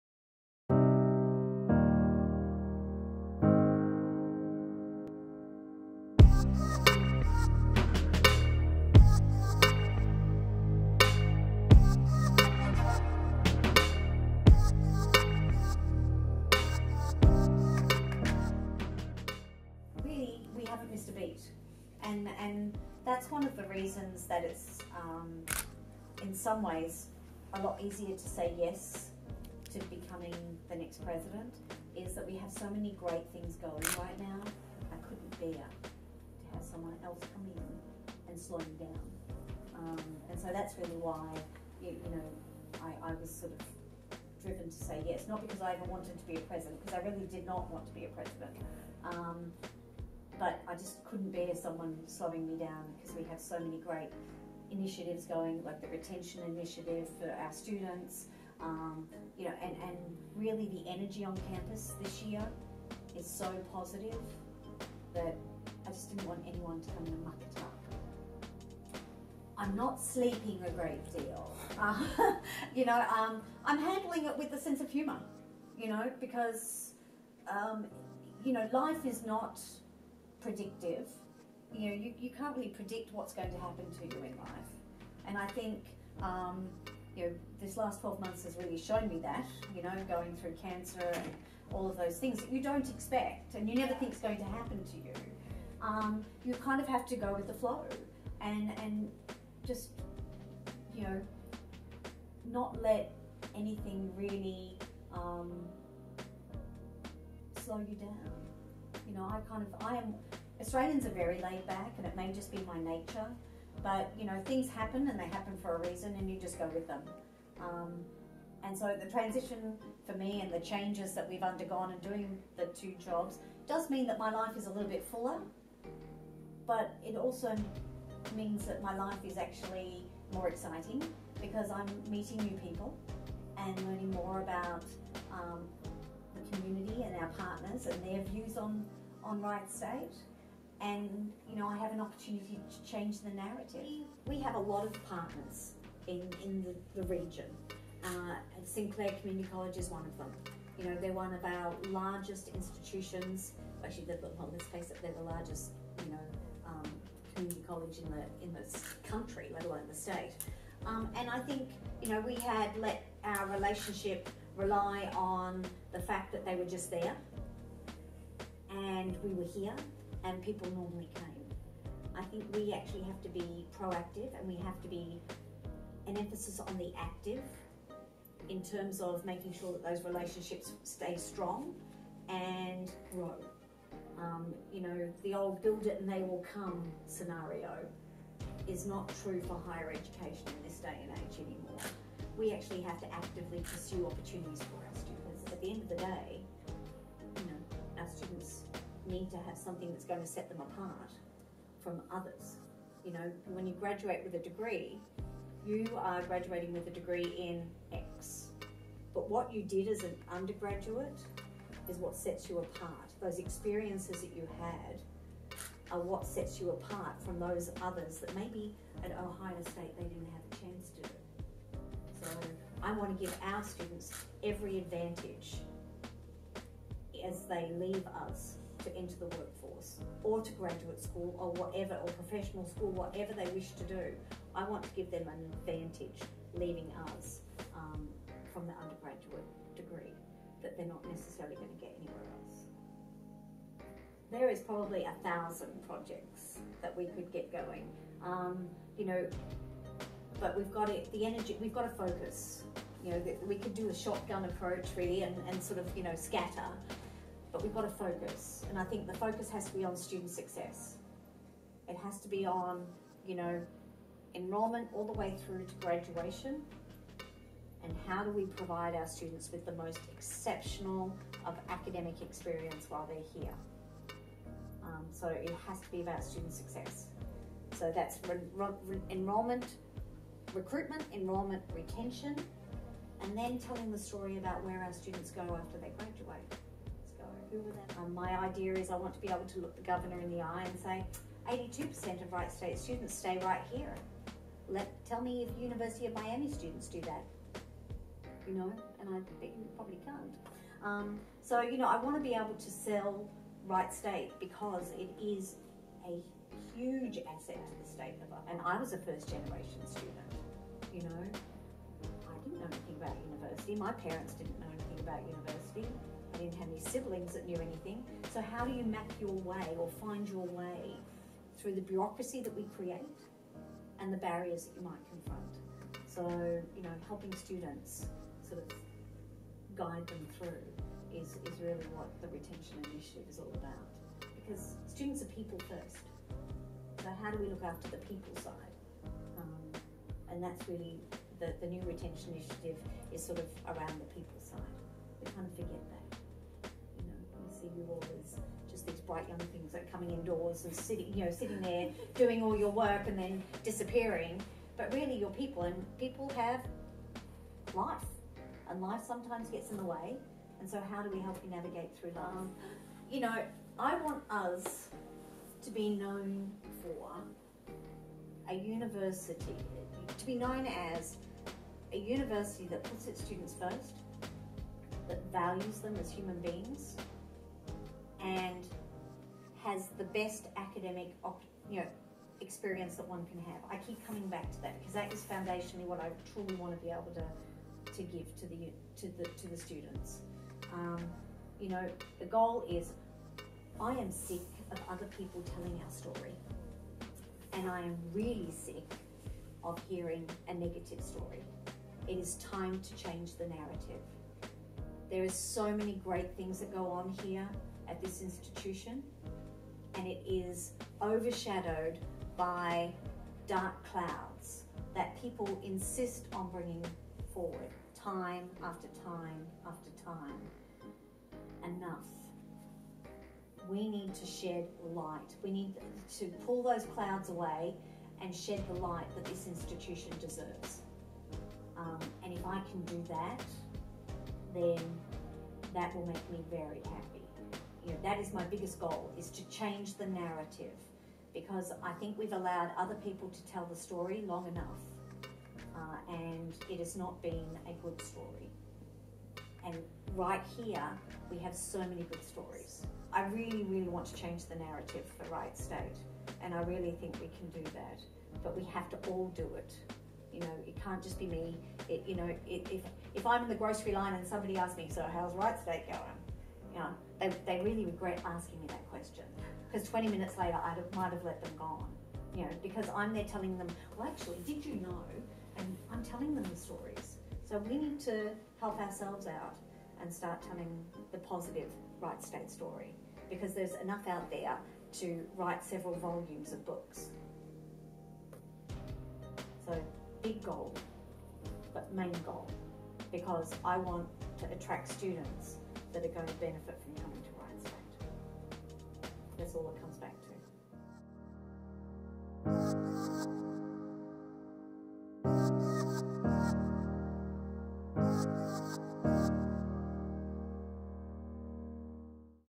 A conversation with incoming president, Susan Edwards
“Getting the role was a surprise but it was a good surprise,” said Susan Edwards, university provost and future university president. “It’s an honor.”

We had the opportunity to sit down Edwards to talk about her transition and goals into the presidency at Wright State.

Edwards will officially become President on Jan. 1, 2020.